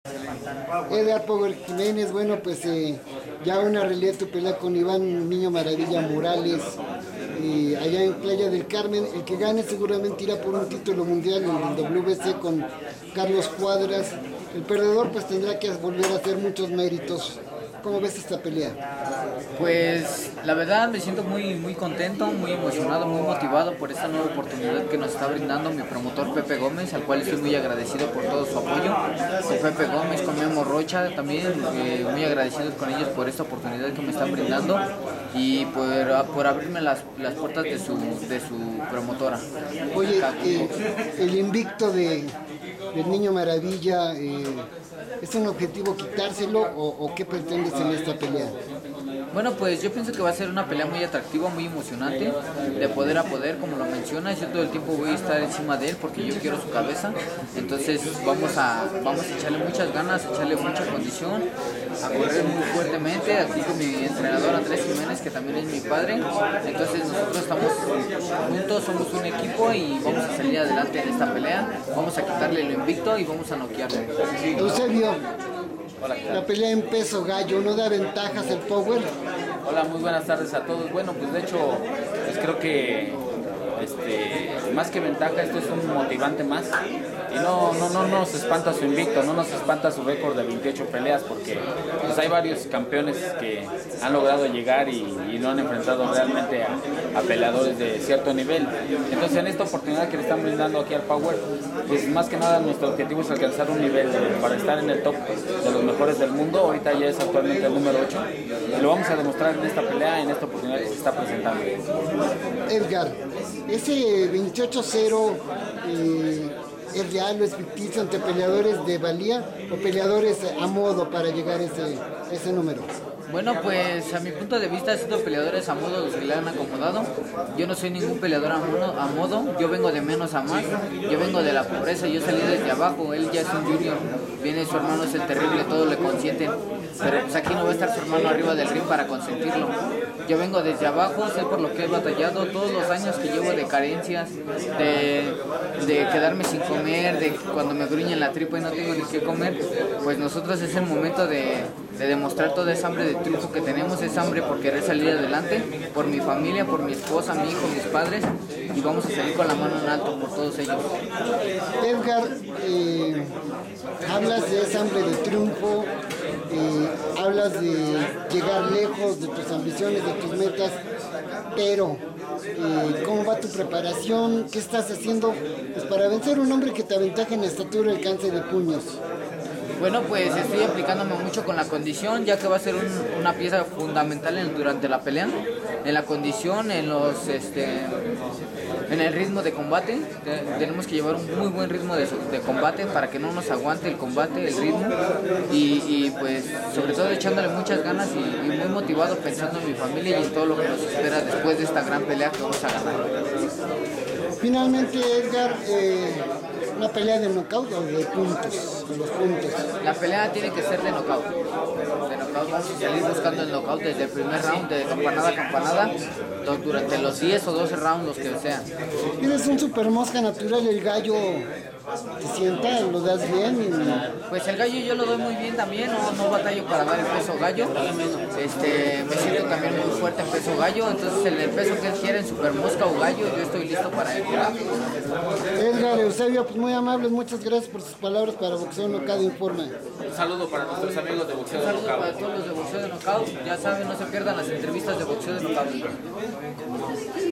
Edgar "Power" Jiménez pelea con Iván "Niño Maravilla" Morales
pelean el 22 de agosto en Playa del Carmen, Quintana Roo